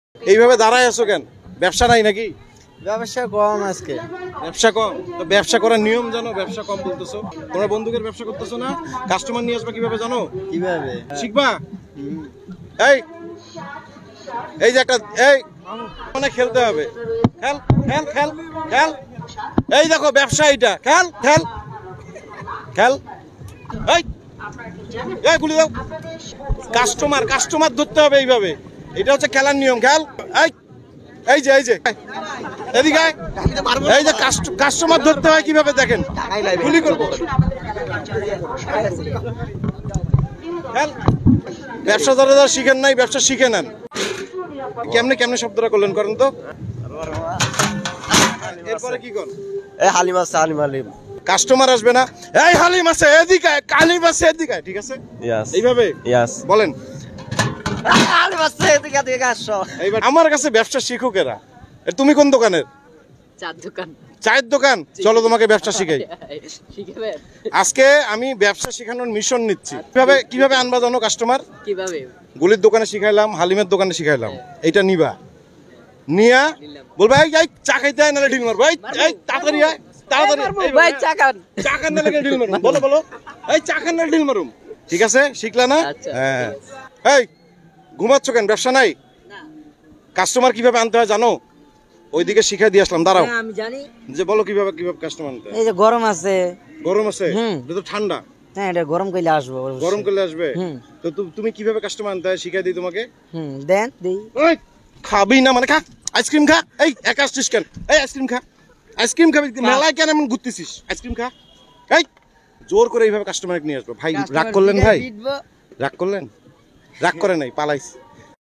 জেনে নিন কিভাবে ব্যবসা বাড়াবেন, দোকানে কাস্টমার না আসলে বা ব্যবসা খারাপ হলে কিভাবে কাস্টমার আনবেন
শিখে রাখুন / জেনে নিন কিভাবে ব্যবসা বাড়াবেন, দোকানে কাস্টমার না আসলে বা ব্যবসা খারাপ হলে কিভাবে কাস্টমার আনবেন... 🤣